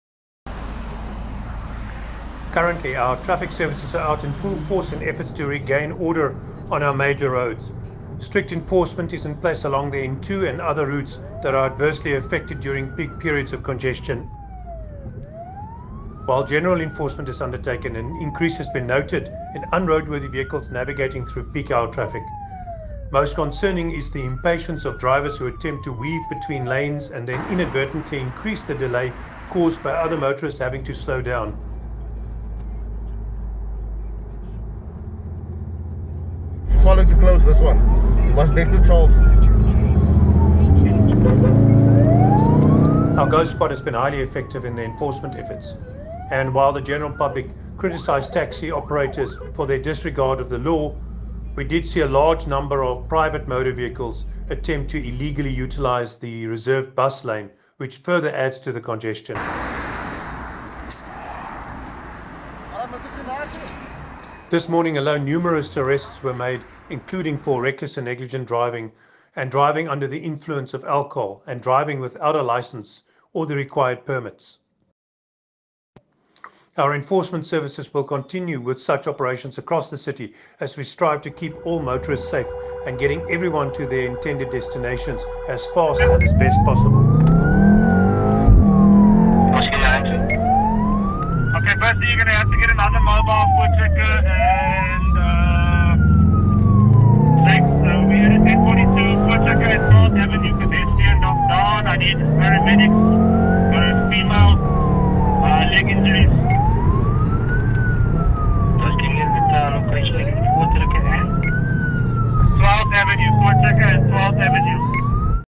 Our Ghost Squad Traffic Officers In Action
While reckless road users often adopt a considerate attitude when passing one of our marked Traffic Services vehicles, trying to identify our unmarked Ghost Squad vehicles is a needle in a haystack.

Sadly for this one taxi driver who attempted to push one of our officers off the road, today was not going to end well when officers discovered the taxi was unlicensed as well as himself not having a license.